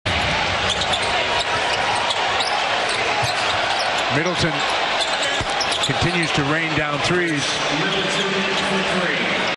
NBA, playoff 2018, Celtics vs. Bucks, Round 1, Game 7, Move 58, Khris Middleton, 3 pointer
Boston Celtics - Milwaukee Bucks, Game 7, 28/04/2018, quarter 4
Khris Middleton, 3 pointer